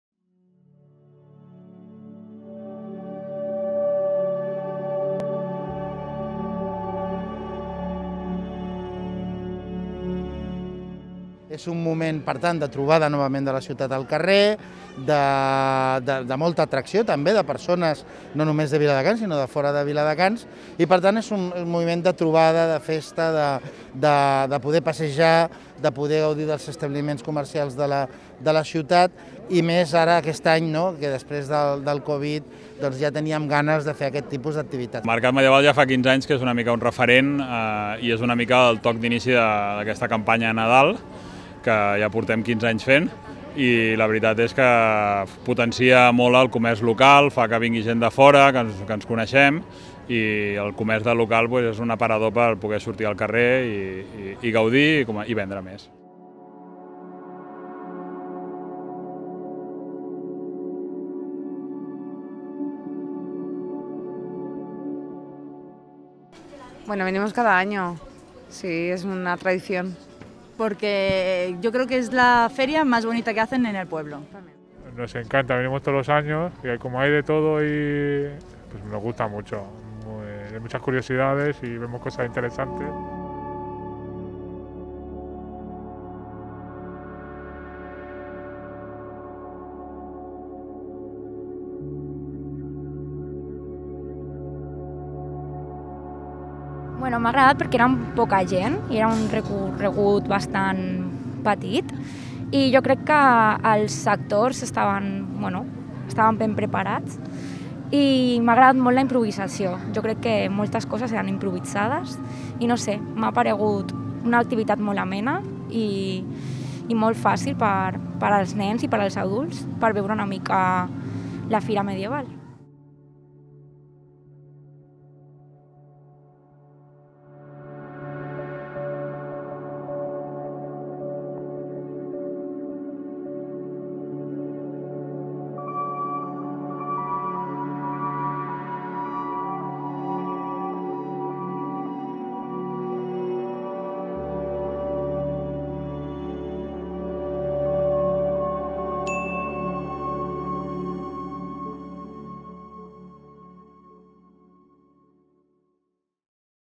Cap de setmana Medieval a Viladecans
Una màquina del temps al passat. Una ja tradició viladecanenca. La 15a Fira Medieval s'ha apoderat de Viladecans aquest cap de setmana.